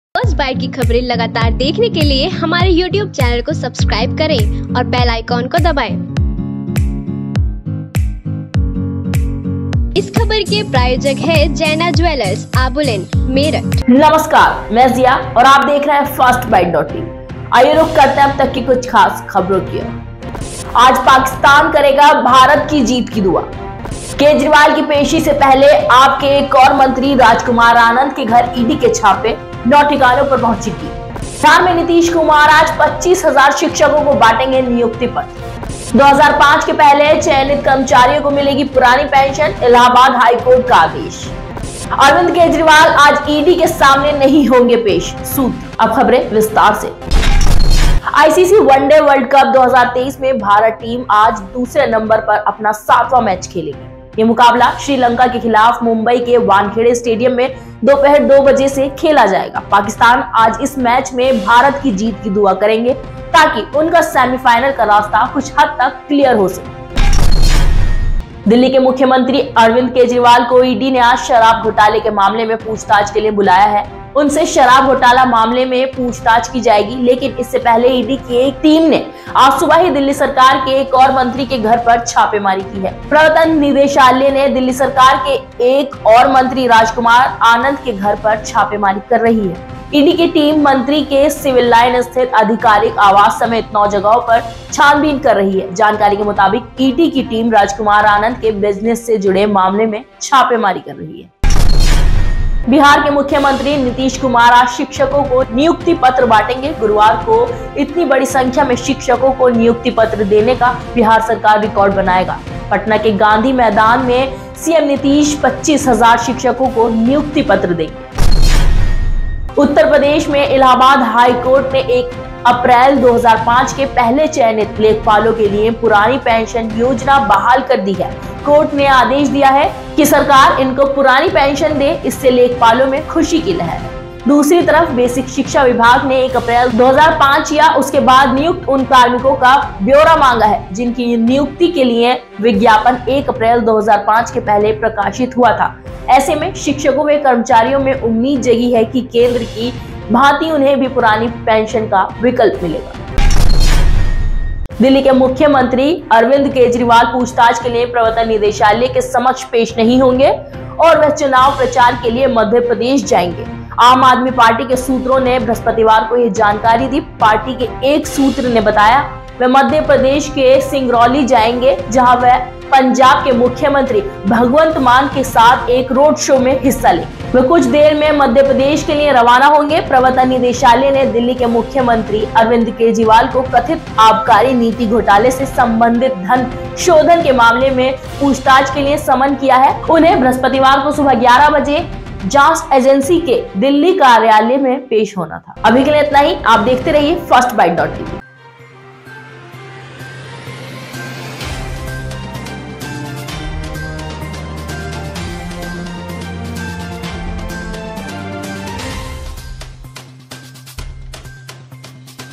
अब तक की कुछ ख़ास ख़बरें । Top 5 । Breaking News - First Byte Tv
आज पाकिस्तान करेगा भारत की जीत की दुआ
केजरीवाल की पेशी से पहले AAP के एक और मंत्री राजकुमार आनंद के घर ED के छापे,
9 ठिकानों पर पहुंची ED कि टीम
बिहार में नीतीश कुमार आज 25000 शिक्षकों को बांटेंगे नियुक्ति पत्र
2005 के पहले चयनित कर्मचारियों को मिलेगी पुरानी पेंशन
इलाहाबाद हाईकोर्ट का आदेश
अरविंद केजरीवाल आज ED के सामने नहीं होंगे पेश : सूत्र